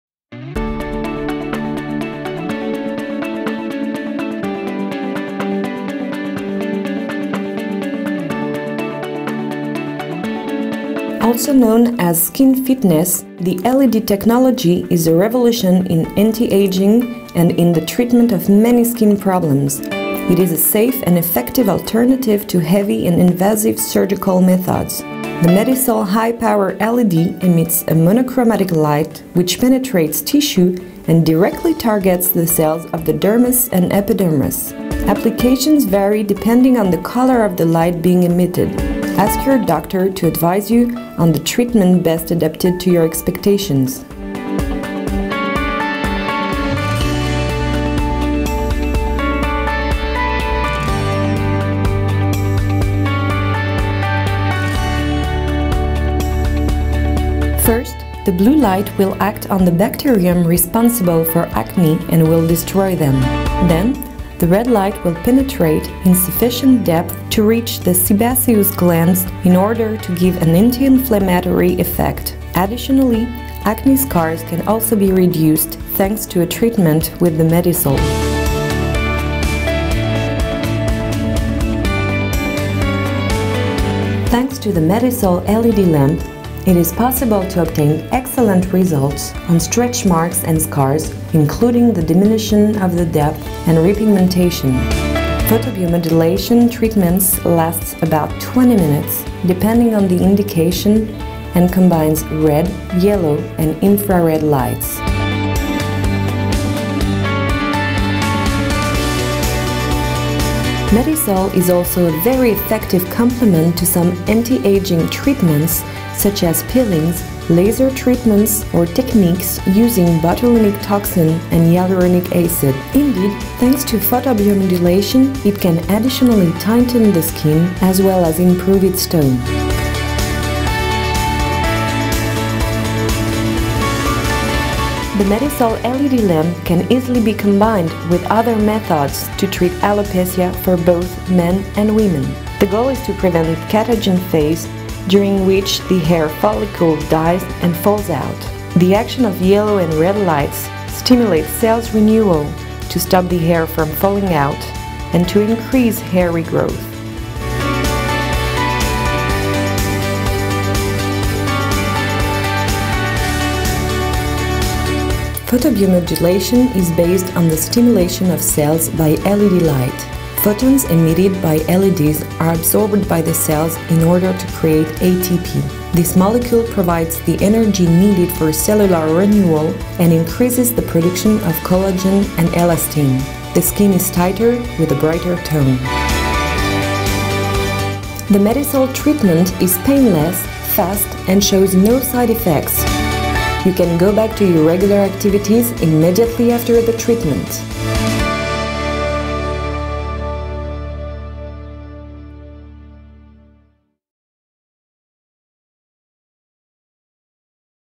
Discover The High Power LED MEDISOL® by Deleo: An In-Depth Presentation
Step into the world of advanced phototherapy with the introduction of the High Power LED MEDISOL® by Deleo, a groundbreaking technology in the realm of aesthetic medicine. This video presentation offers a comprehensive overview of MEDISOL®'s capabilities, showcasing how it leverages high-power LED technology to provide effective, non-invasive treatments for a variety of skin conditions and concerns.

MEDISOL® by Deleo is designed to deliver targeted photobiomodulation therapy, utilizing specific wavelengths of light to promote skin healing, rejuvenation, and improve overall skin appearance. This detailed presentation explains the science behind LED therapy, the unique features of MEDISOL®, and how it stands out from other phototherapy devices in terms of power, efficacy, and versatility.

Video Highlights:

Introduction to High Power LED MEDISOL®: Learn about the cutting-edge MEDISOL® device and its significance in non-invasive aesthetic treatments.
The Science of LED Phototherapy: Delve into the principles of LED phototherapy and how MEDISOL® utilizes specific light wavelengths to target different skin issues effectively.
Unique Features and Benefits: Explore the standout features of MEDISOL®, including its high power output, customizable treatment protocols, and the wide range of skin conditions it can address.
Clinical Applications and Results: Discover the diverse applications of MEDISOL® in clinical practice, from acne and inflammation reduction to skin rejuvenation and post-procedure healing. View before-and-after images showcasing the transformative results achieved with MEDISOL® treatments.
Safety and Patient Experience: Understand the safety profile of MEDISOL® treatments and the positive patient experience it offers, including comfort during treatments and lack of downtime.
This video is an invaluable resource for aesthetic professionals seeking to enhance their treatment offerings with the latest in LED phototherapy technology, as well as for individuals curious about the benefits of non-invasive skin treatments. With the High Power LED MEDISOL® by Deleo, discover a new standard in phototherapy that promises to revolutionize the approach to skin care and rejuvenation.

If you're intrigued by the potential of the High Power LED MEDISOL® by Deleo and wish to learn more, please like, share this video, and leave your comments or questions below. Embrace the future of aesthetic medicine with the innovative MEDISOL® system.